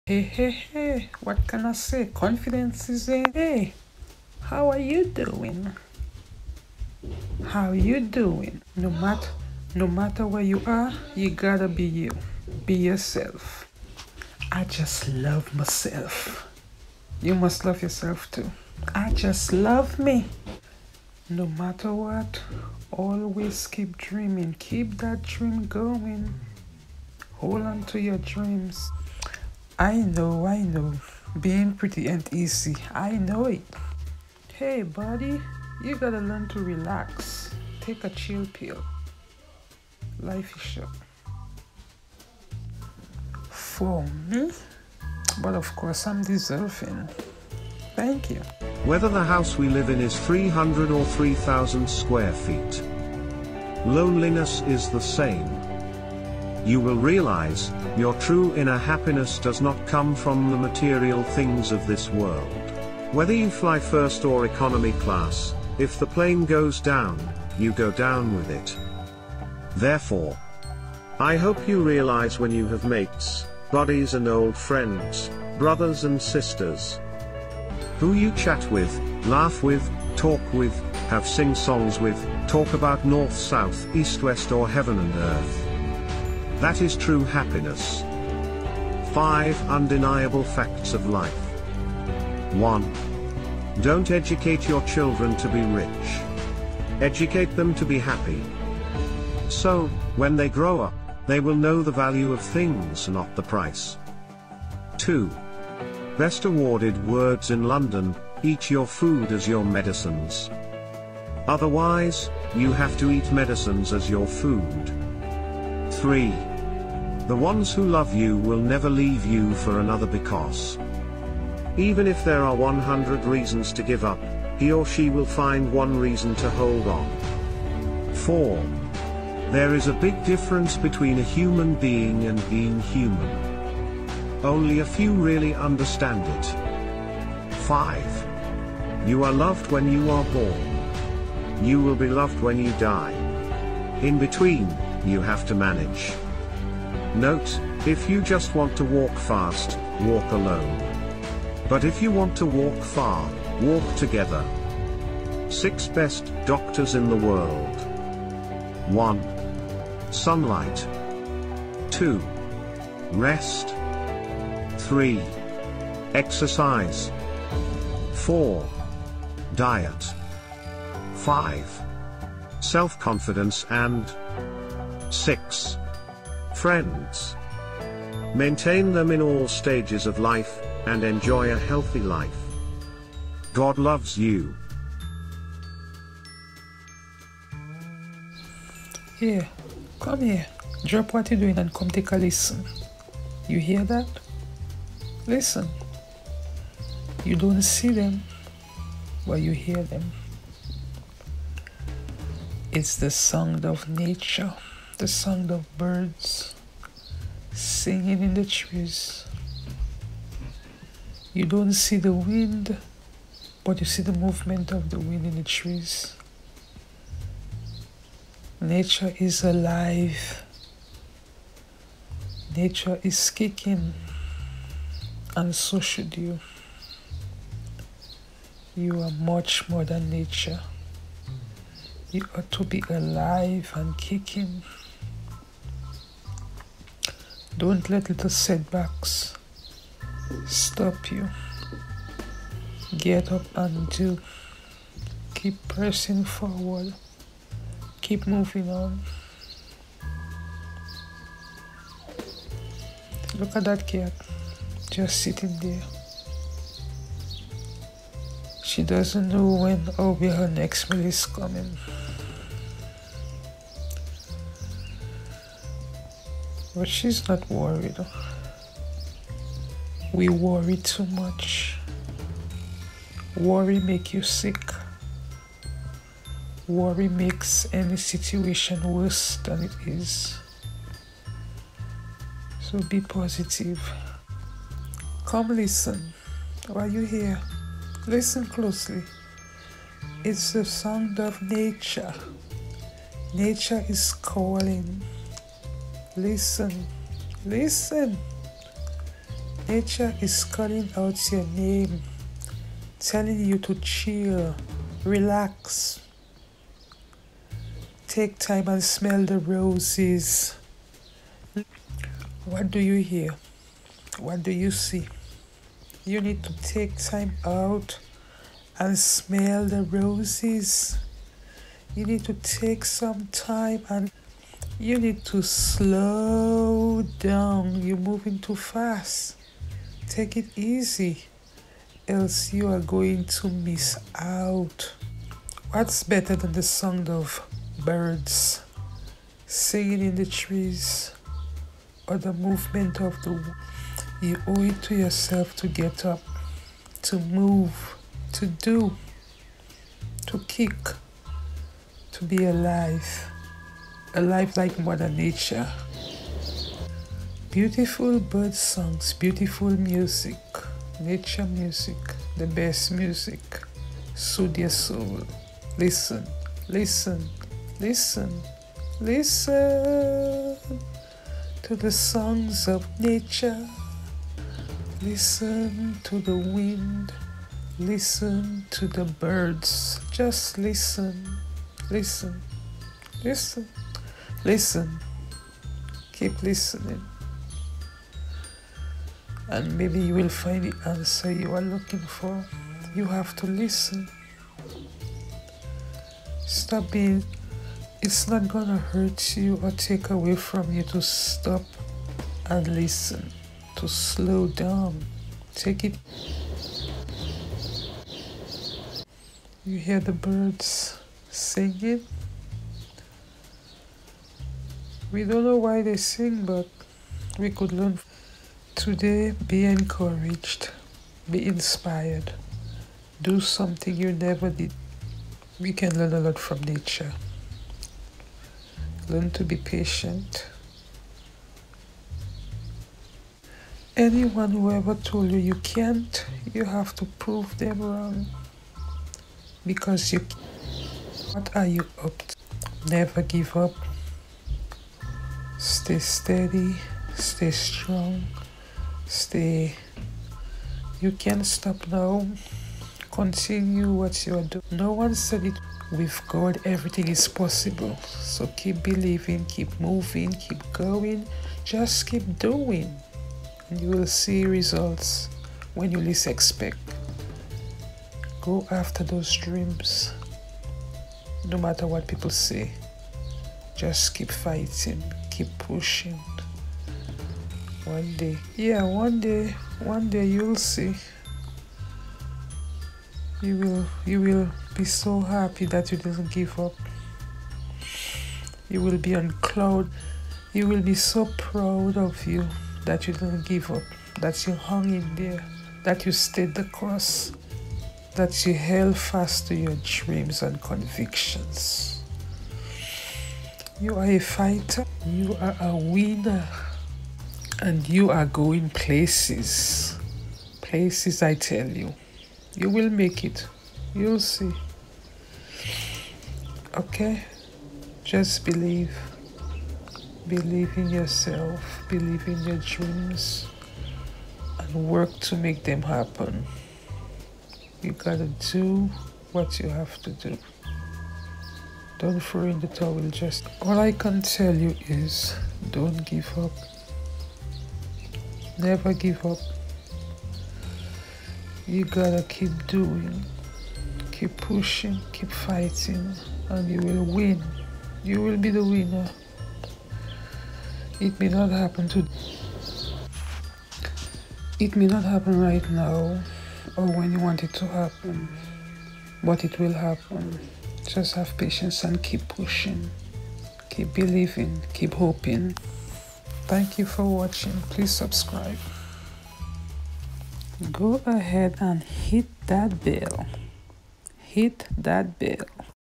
Learn From Nature | Motivate Yourself | nature motivational video
Learn from nature. Motivate yourself each day to accomplish your goals. Continue doing all you can to make those dreams come true and they will happen. Don't allow the negative opinions of others to cloud your goals and aspirations. Practice positive I AMs , learn from nature and keep working to make it happen.